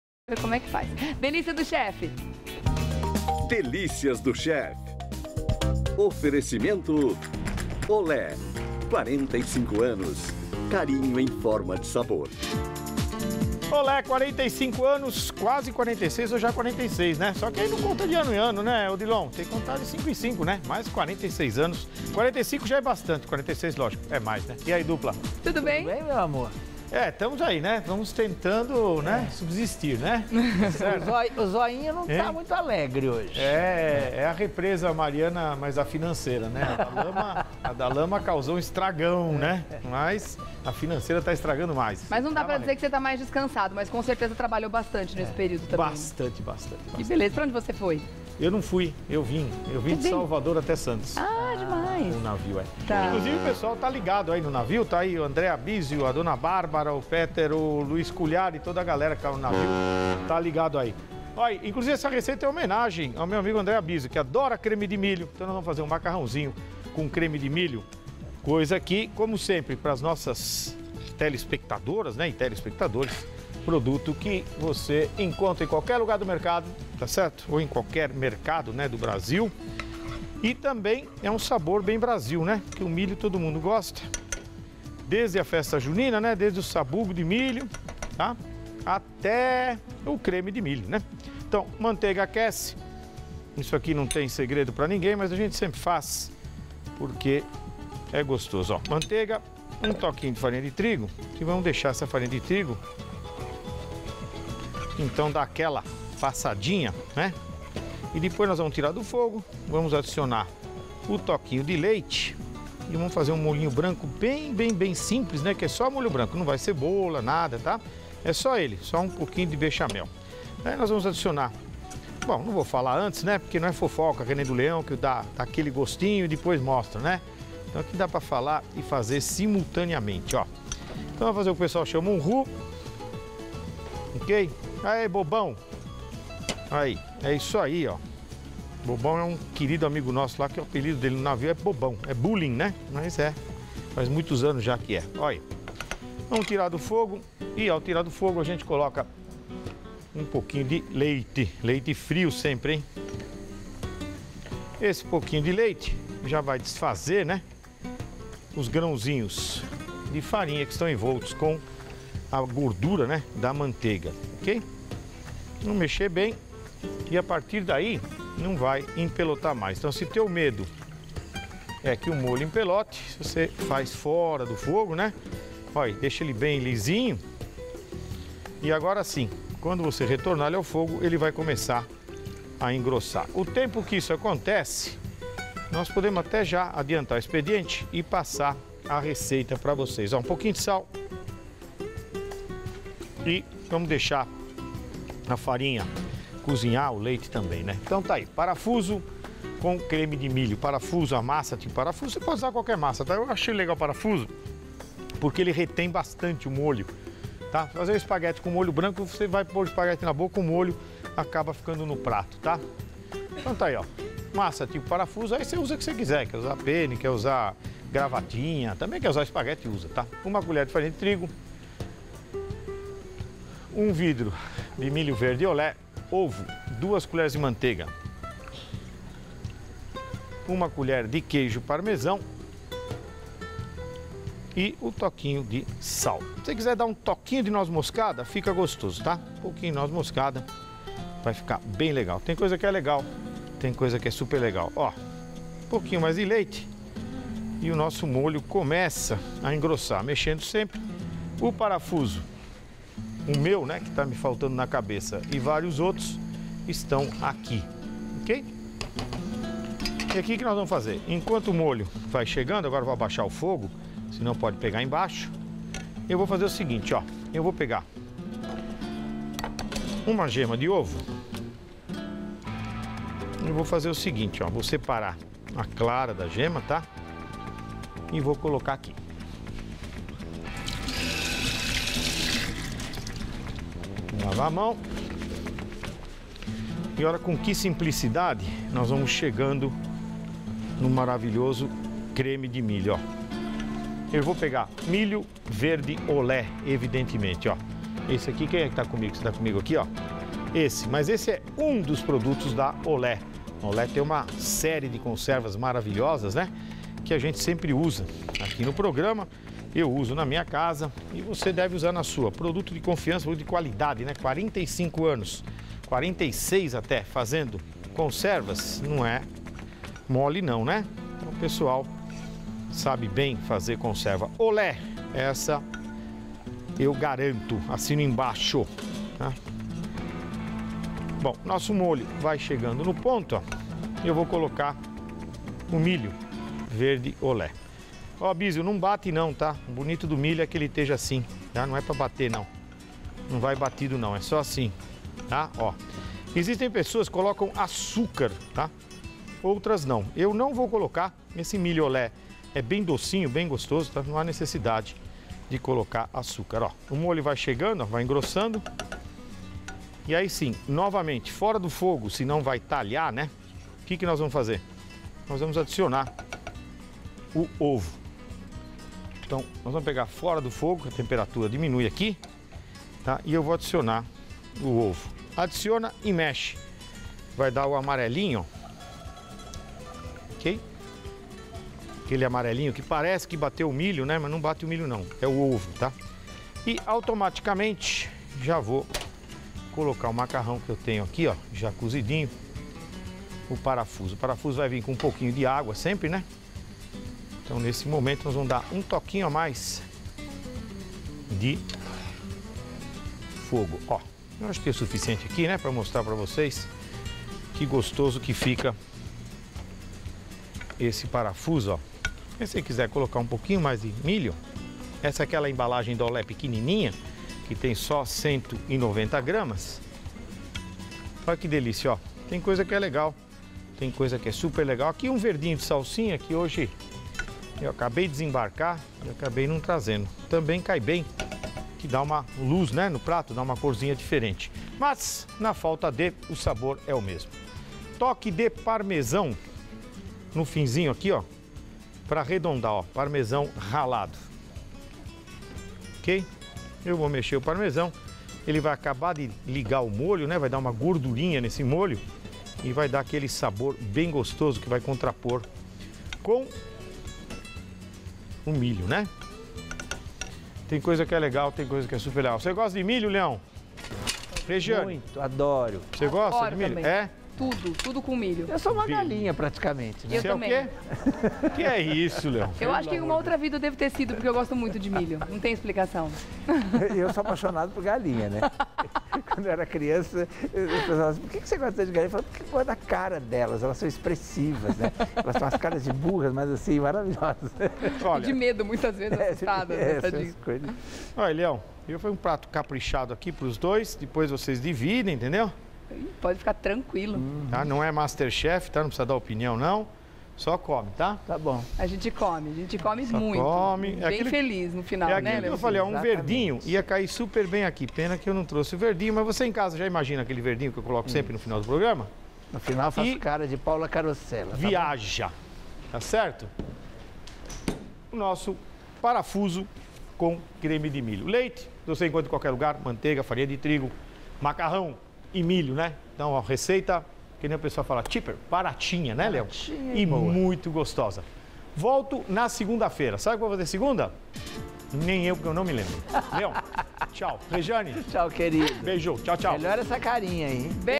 Revista da Cidade - Delícias do chef: Parafuso com creme de milho (18/11/15)
Aprenda a fazer um delicioso Parafuso com creme de milho.